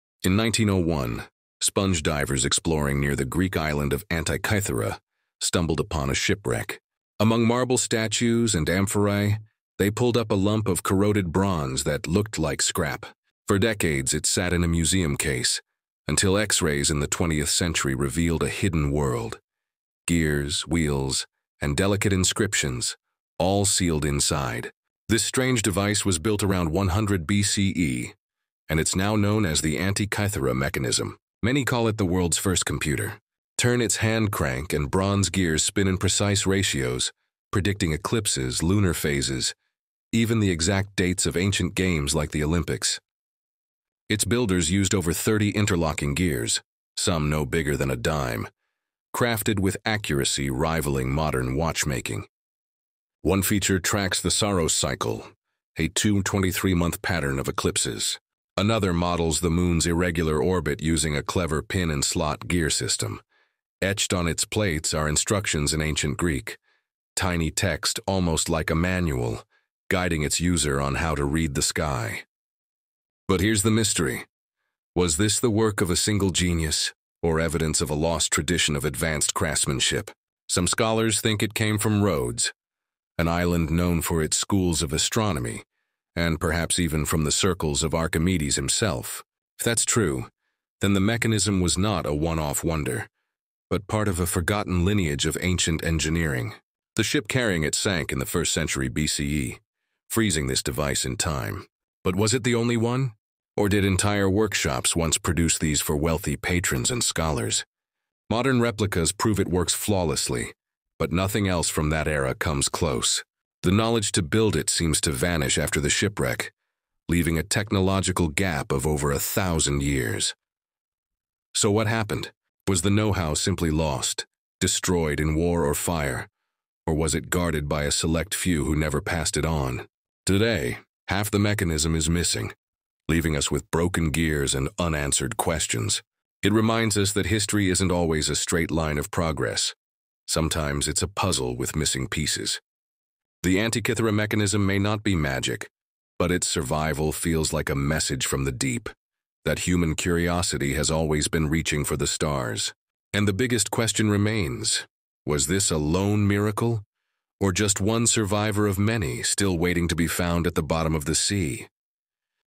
The Antikythera Mechanism: The World’s First Computer?
The Antikythera Mechanism may be the world’s first computer—an ancient device lost to time and mystery. How could technology like this exist thousands of years ago?